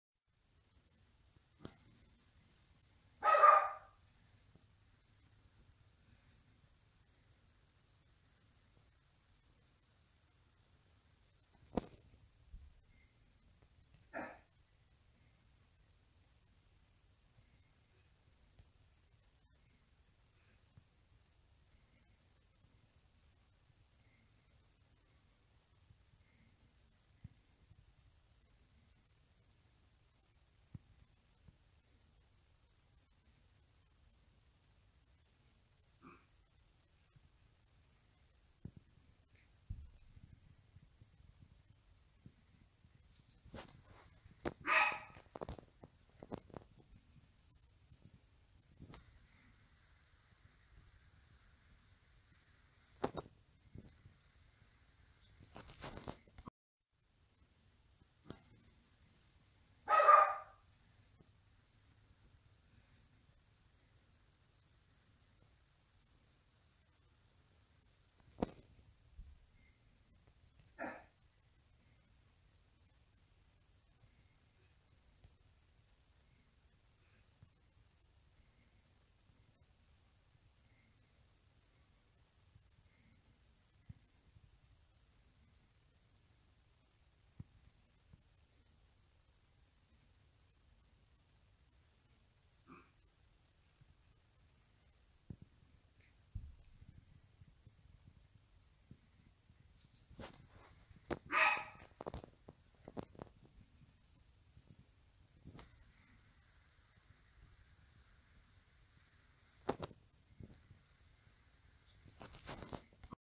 aliens behind fence in my creek
look carefully a little bit of glare but there is unusual things going on my fogs are going nuts one looks threw the gate and to the side